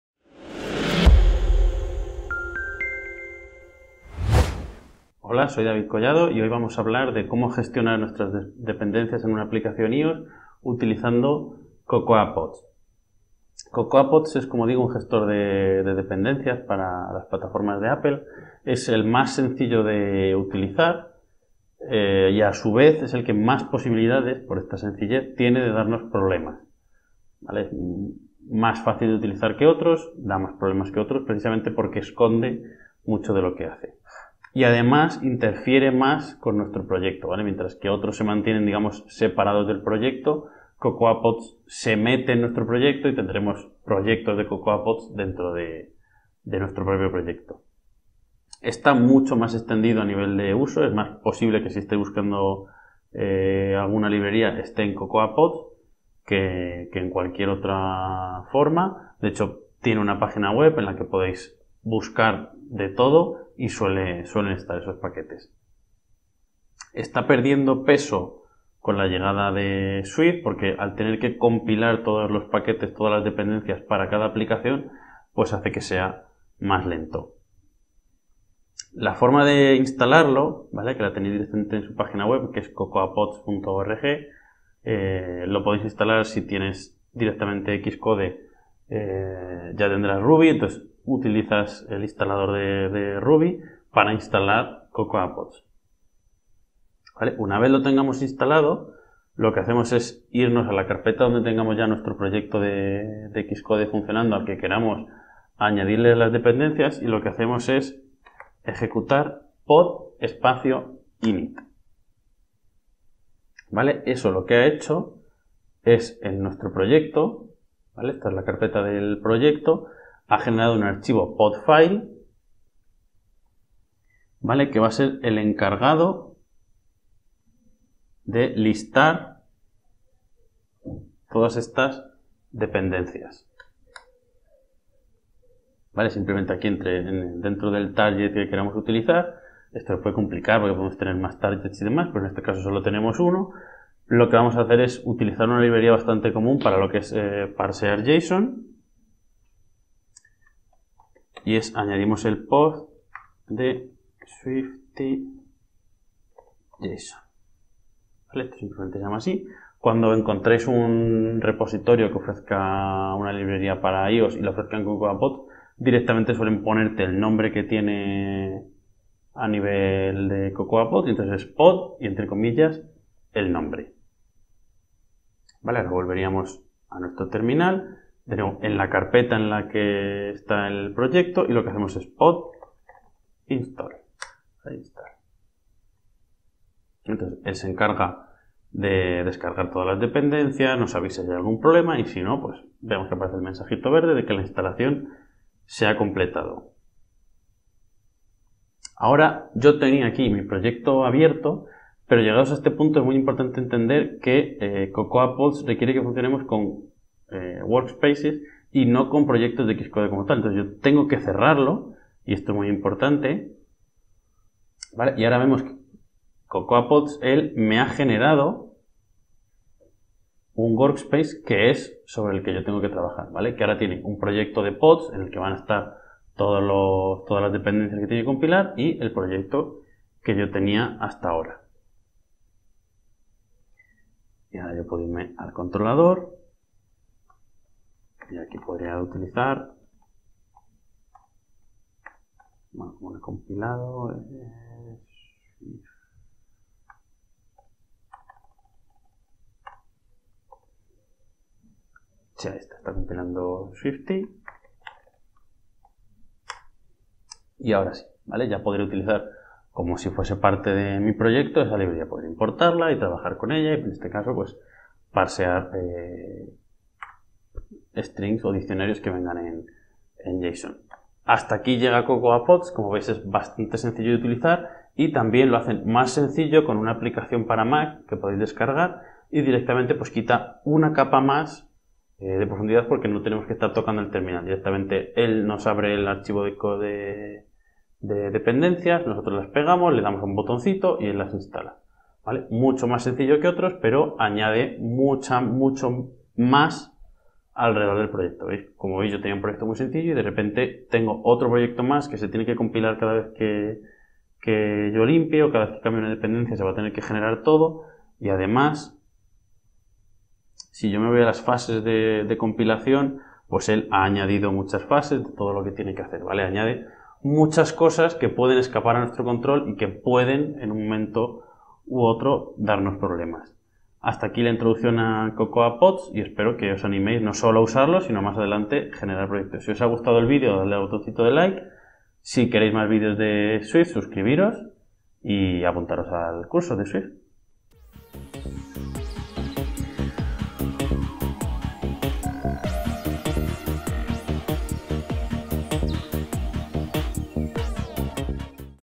GESTIONAR DEPENDENCIAS CON COCOAPODS
En esta ocasión vamos a ver cómo funciona Cocoapods, un gestor de dependencias para nuestros proyectos de Xcode.